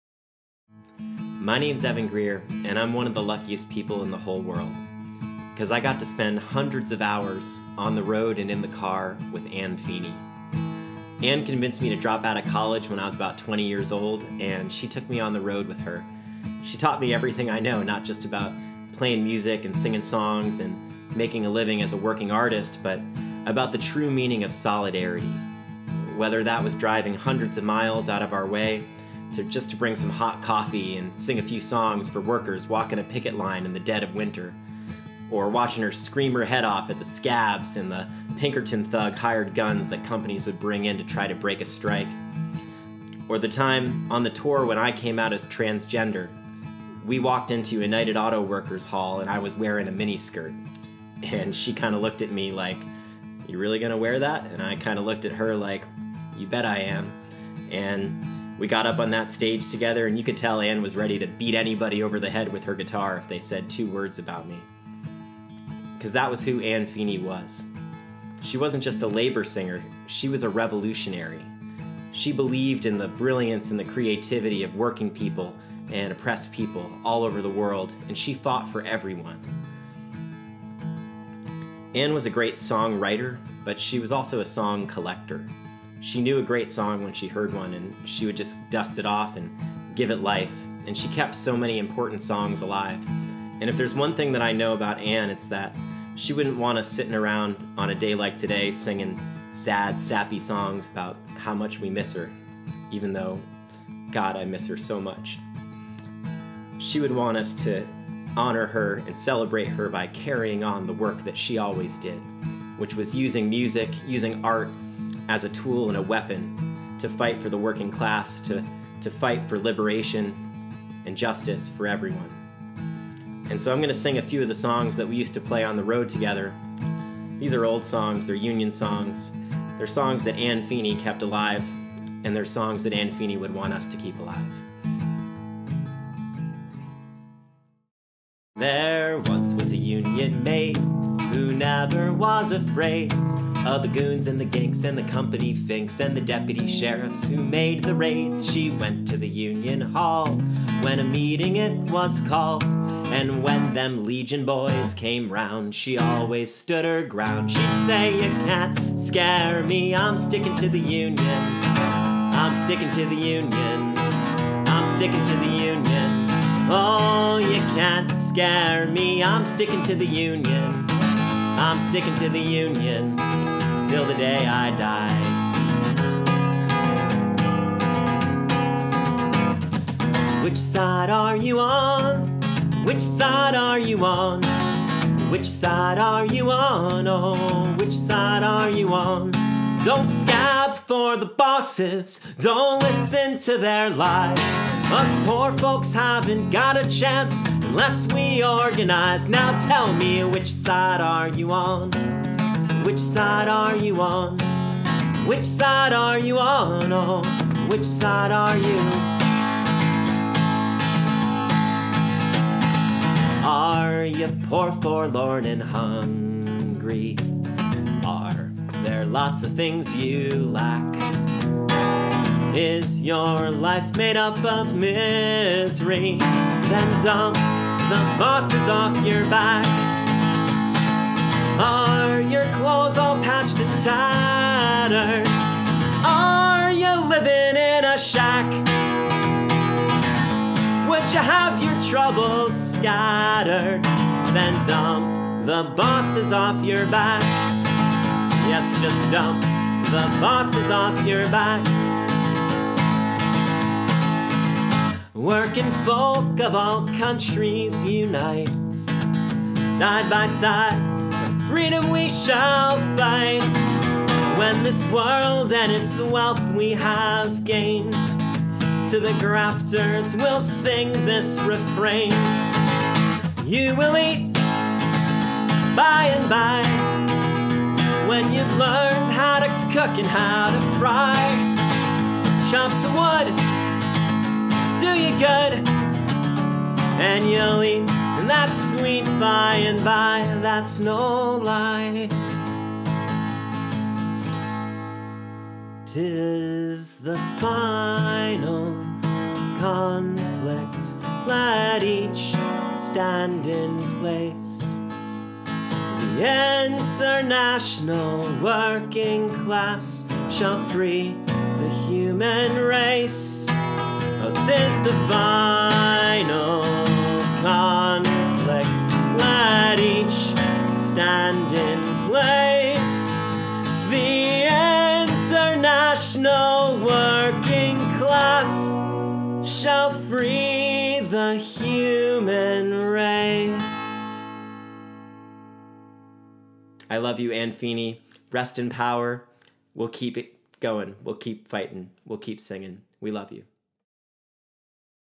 Evan Greer tribute to Anne Feeney
Here's the video I recorded for the virtual memorial we organized for my dear friend, touring partner and mentor Anne Feeney.

You can watch the whole program, and donate to the Anne Feeney Hellraiser Memorial Fund,